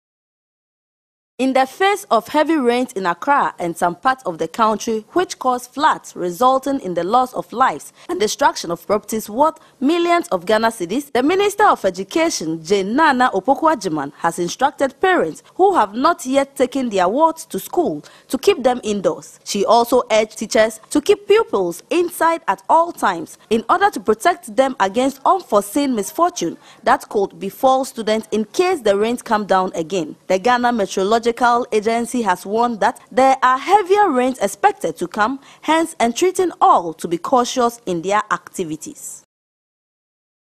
MINISTER OF EDUCATION WORNS BOTH PARENT AND TEACHERS TO KEEP CHILDREN INDOORS
News Edge - MINISTER OF EDUCATION WORNS BOTH PARENT AND TEACHERS TO KEEP CHILDREN INDOORS